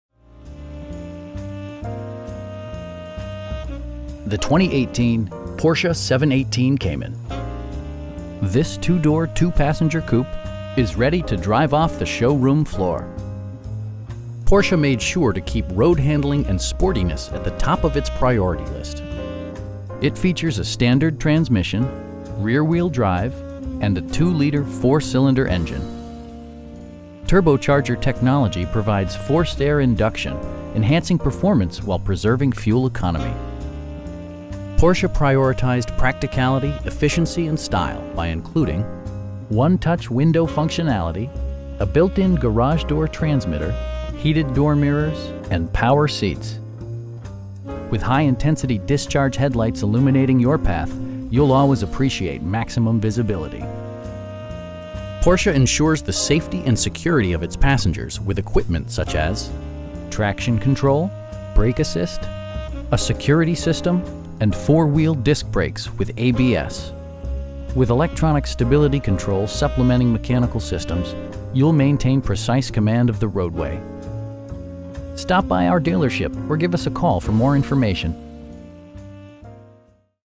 2018 Porsche 718 Cayman Base in Wilmington, NC 28403
BMW of Wilmington
4920 New Centre Dr

The 2018 Porsche 718 Cayman. This 2 door, 2 passenger coupe is ready to drive off the showroom floor! Porsche made sure to keep road-handling and sportiness at the top of it's priority list. It features a standard transmission, rear-wheel drive, and a 2 liter 4 cylinder engine. Turbocharger technology provides forced air induction, enhancing performance while preserving fuel economy. Porsche prioritized practicality, efficiency, and style by including: 1-touch window functionality, a built-in garage door transmitter, front bucket seats, heated door mirrors, power windows, cruise control, and power seats. With high intensity discharge headlights illuminating your path, you'll always appreciate maximum visibility. Porsche ensures the safety and security of its passengers with equipment such as: dual front impact airbags, head curtain airbags, traction control, brake assist, a security system, and 4 wheel disc brakes with ABS. With electronic stability control supplementing mechanical systems, you'll maintain precise command of the roadway. We pride ourselves in consistently exceeding our customer's expectations. Stop by our dealership or give us a call for more information.